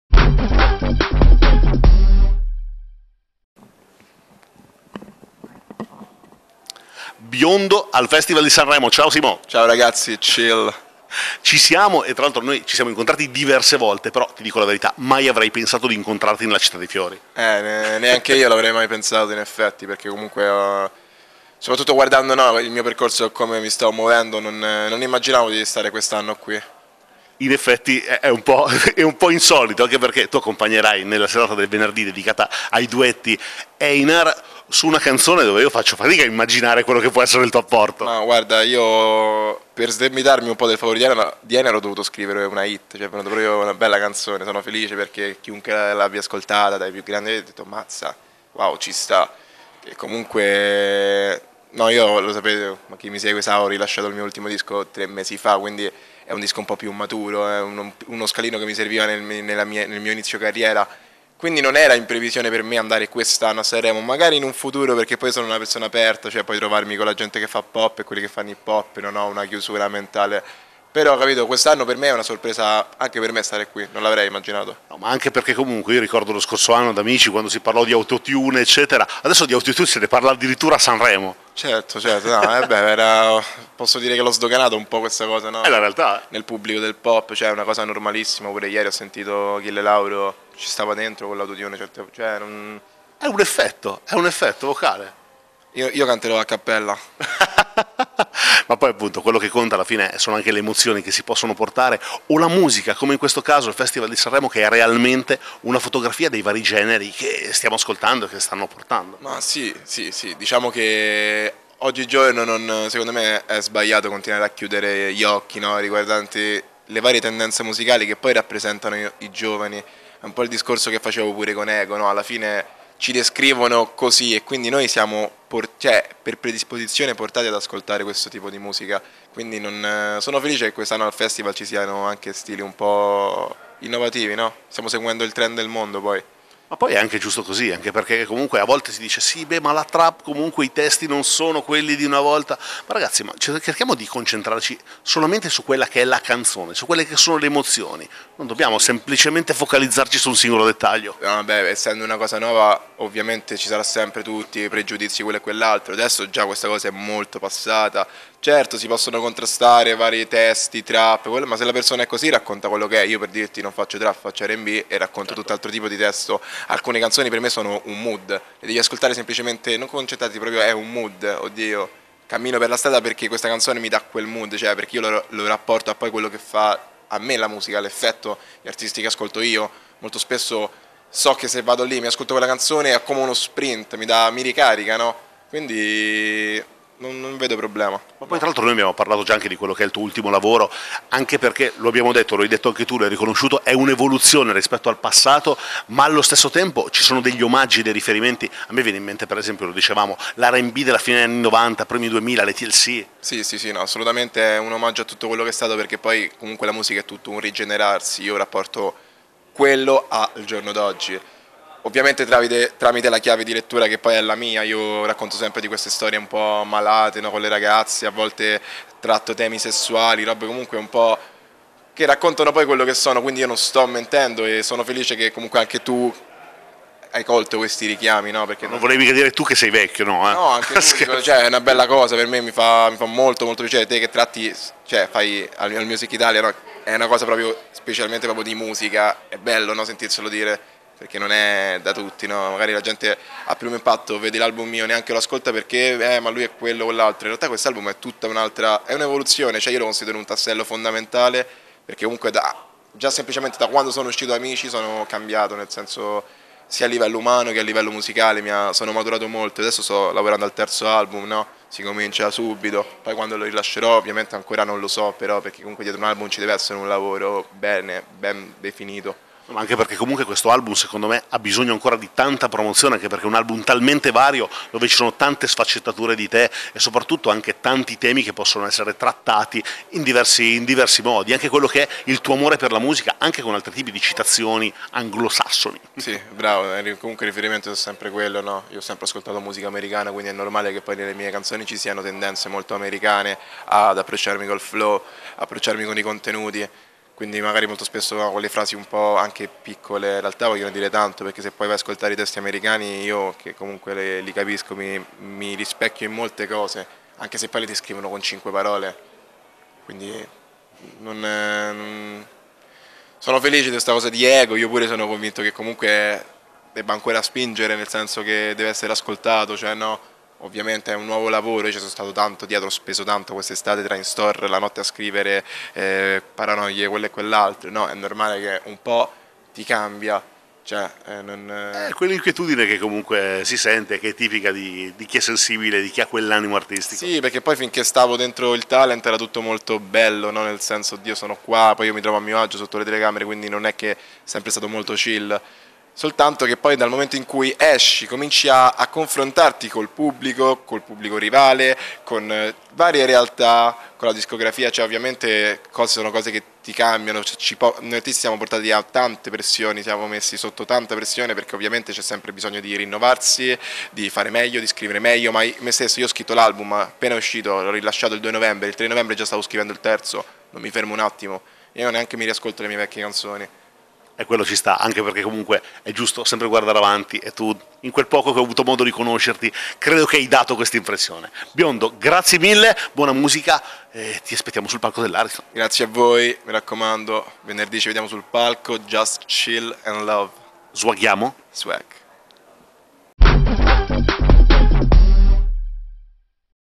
Biondo intervista da Sanremo 2019
Biondo intervista Sanremo 2019

Il giovane Biondo quest'anno salirà sul palco del Festival di Sanremo 2019 per affiancare nella serata dei duetti il suo amico, ed ex compagno ad Amici di Maria De Filippi, Einar. All Music Italia lo ha intervistato per farsi raccontare le ultime novità che lo riguardano...